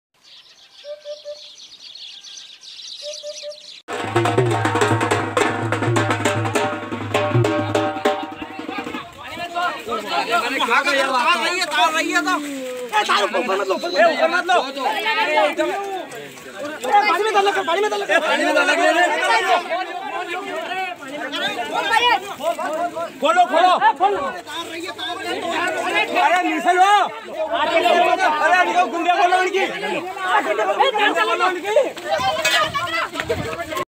This Shantilal Dhangar, resident of Barod village of Sitamau, Tehsil of Mandsaur, who is the young m
This Shantilal Dhangar, resident of Barod village of Sitamau, Tehsil of Mandsaur, who is the young member of RSS, was holding a rally to show happiness to all after Ram Mandir's Bhoomi Pooja and forgot in the spirit that the electric wire above  In which, when he touched the iron pipe in his flag, he died due to current shock.  RIP